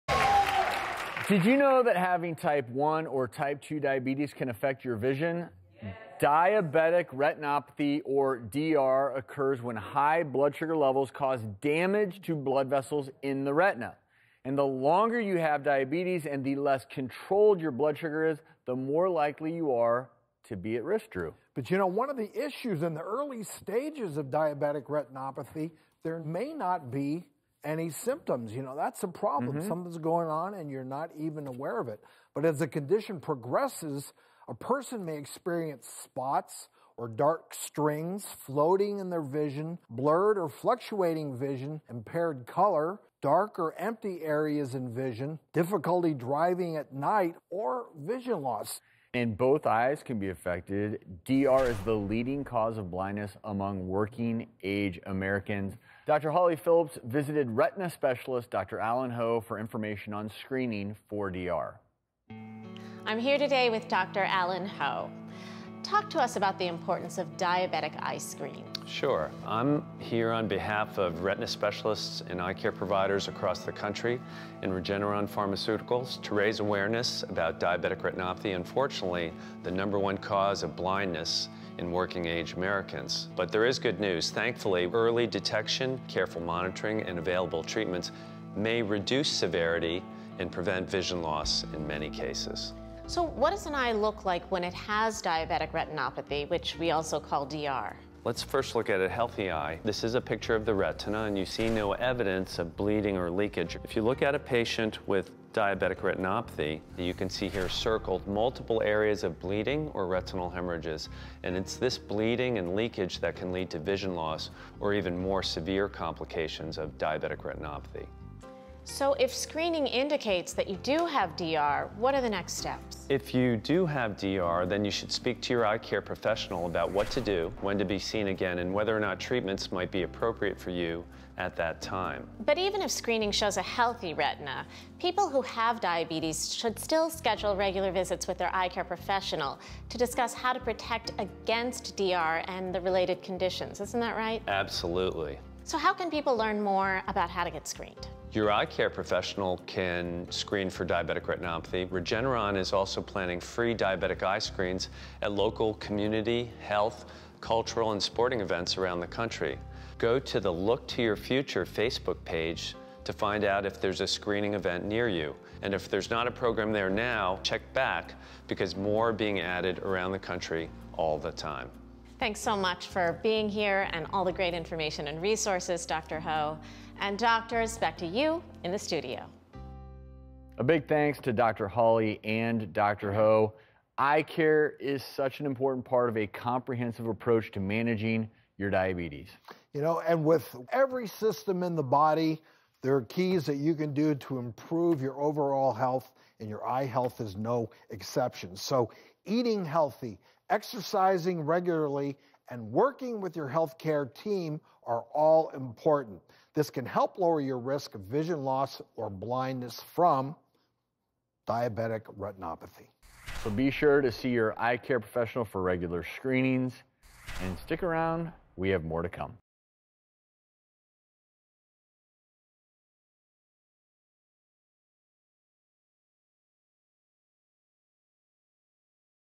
Diabetes & Vision: What One Needs to Know
Did you know that having type 1 or type 2 diabetes can affect one's vision? Retina specialist & Regeneron spokesperson Dr. Allen Ho explains the signs, symptoms & treatment of diabetic retinopathy (DR), the number one cause of blindness in working-age Americans.